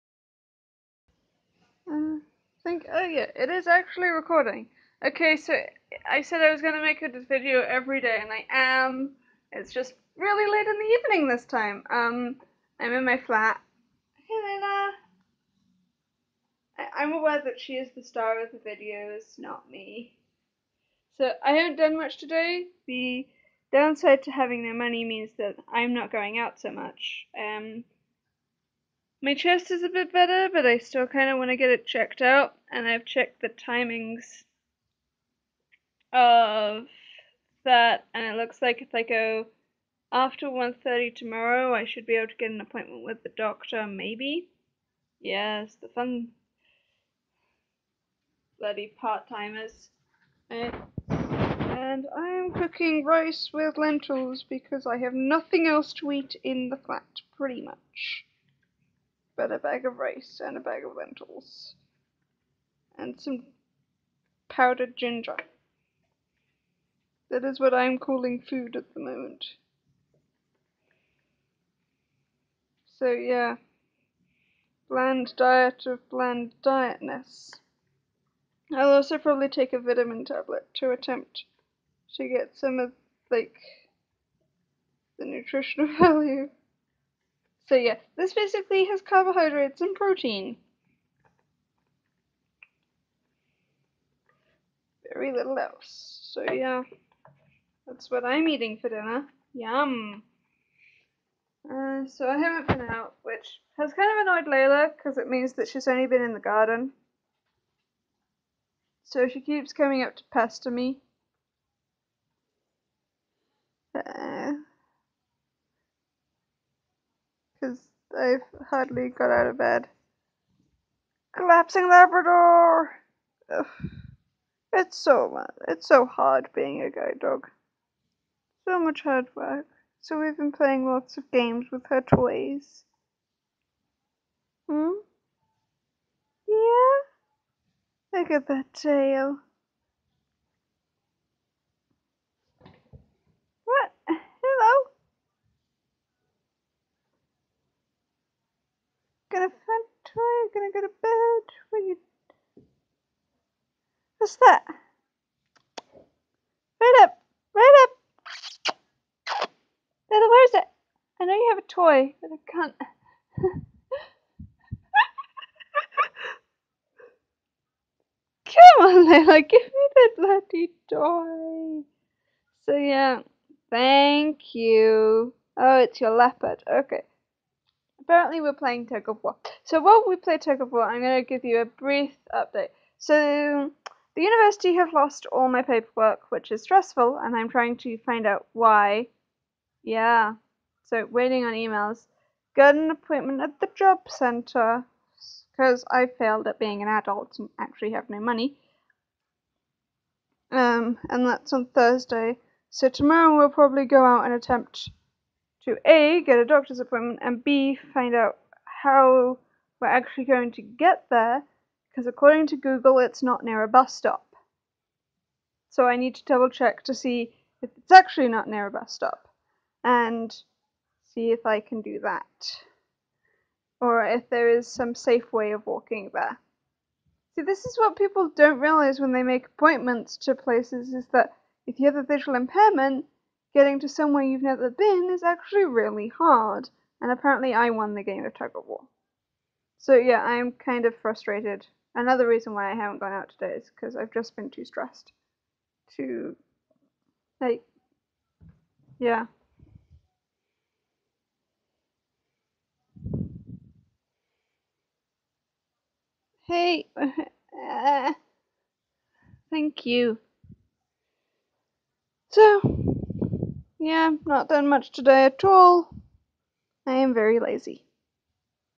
talking about why being a vi is frustrating
sometimes people who can see can really be frustrating
also playing tug of war with layla
and cooking